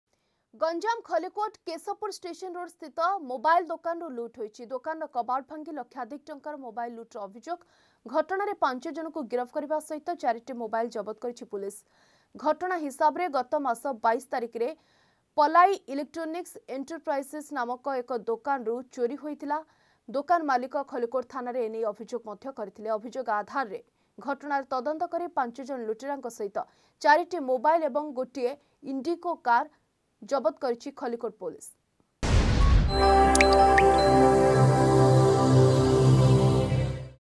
ଗଞ୍ଜାମ ଖଲିକୋଟରେ ମୋବାଇଲ ଦୋକାନରୁ ଲୁଟ୍ ....
ଗଞ୍ଜାମ ଖଲିକୋଟ କେଶପୁର ଷ୍ଟେସନ ରୋଡସ୍ଥିତ ମୋବାଇଲ ଦୋକାନରୁ ଲୁଟ୍ । ଦୋକାନର କବାଟ ଭାଙ୍ଗି ଲକ୍ଷାଧିକ ଟଙ୍କାର ମୋବାଇଲ ଲୁଟ୍ ଅଭିଯୋଗ । ଏହି ଘଟଣାରେ 5ଜଣଙ୍କୁ ଗିରଫ କରିବା ସହ 4ଟି ମୋବାଇଲ ଜବତ କରିଛି ପୋଲିସ .